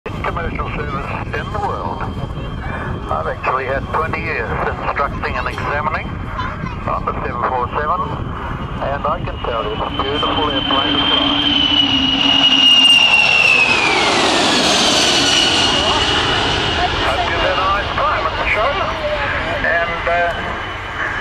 747 Makes a low high speed pass
Boeing 747 airliner makes a high speed, low flyby pass. Aeroplane is also known as the Jumbo Jet.